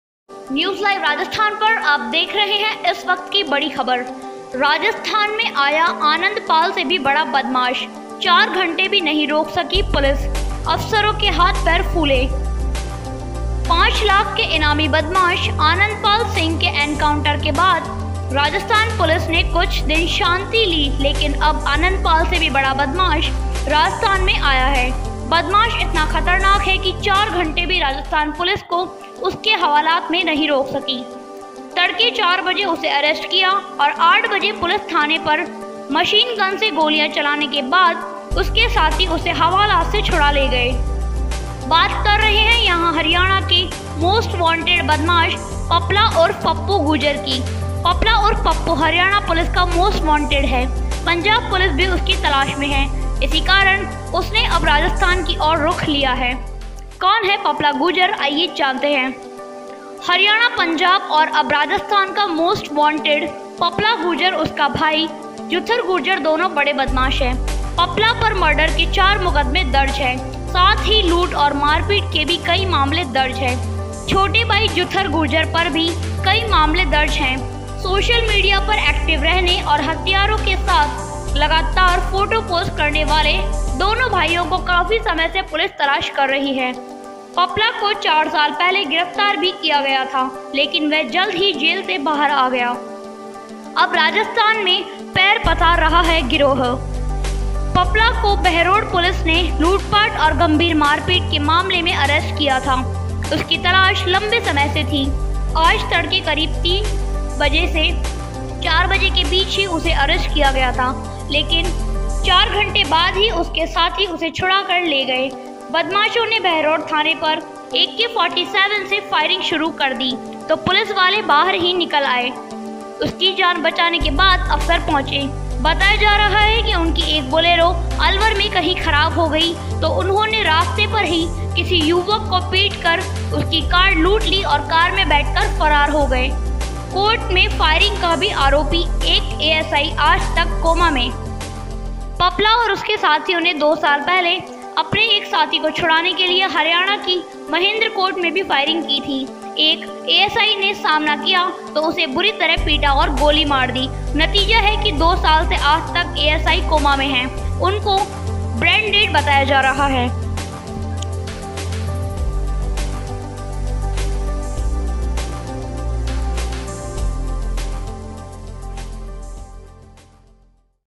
News Live Rajasthan राजस्थान में आया आनंदपाल से भी बड़ा बदमाश 4 घंटे भी नहीं रोक पाई पुलिस अफसरों के
News Live Rajasthan राजस्थान में आया आनंदपाल से भी बड़ा बदमाश 4 घंटे भी नहीं रोक पाई पुलिस अफसरों के हाथ पैर फुले